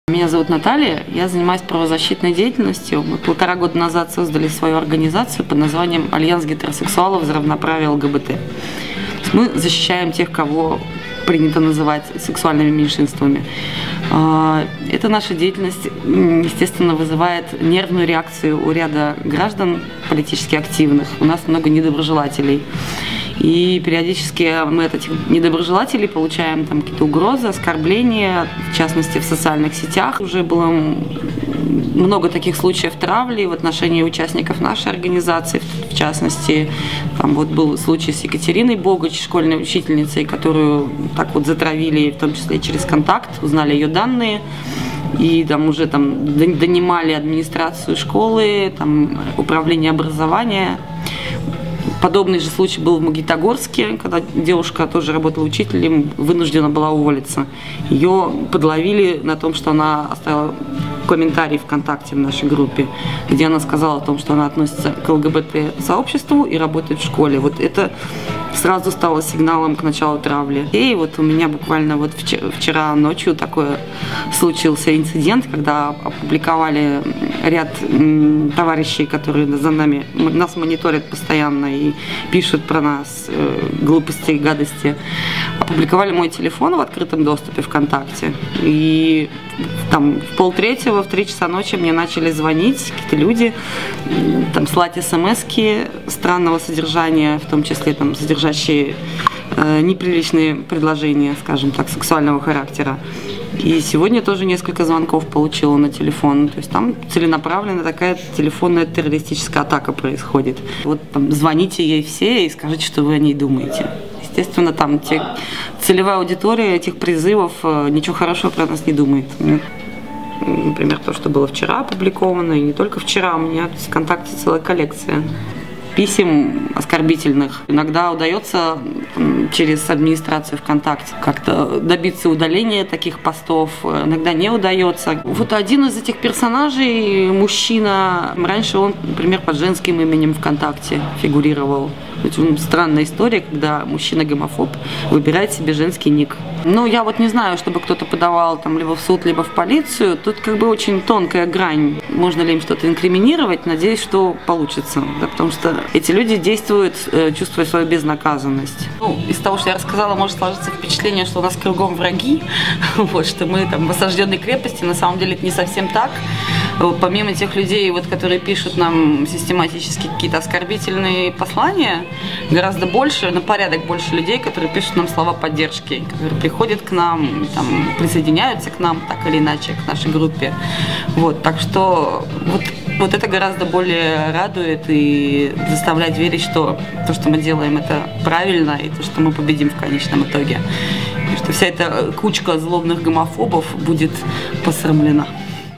В Петербурге угрожают защитнице прав ЛГБТ
Наталья Цымбалова, координатор "Альянса гетеросексуалов за равноправие ЛГБТ" в Петербурге, за последние дни получила множество угроз и оскорблений расистского и гомофобного характера. Травля началась после недавней акции  "Берлин 1936 = Сочи 2014".